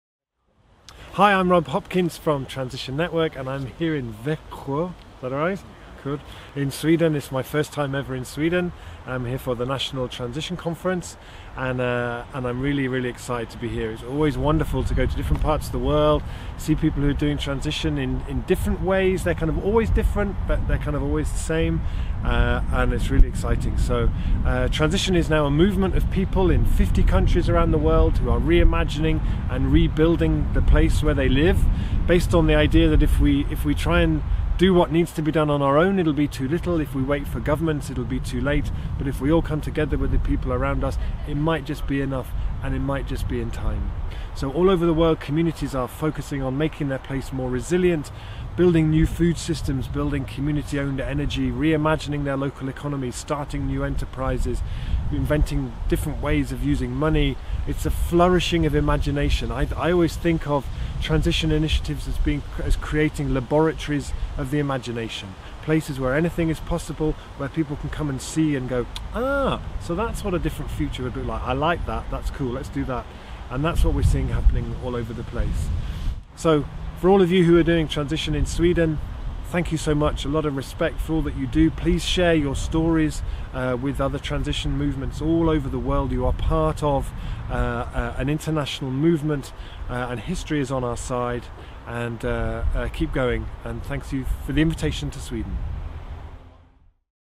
Rob Hopkins in Sweden
Rob Hopkins, founder of the Transition movement, visiting the Swedish national Transition Network conference, held in Växjö 7-8 October, 2017.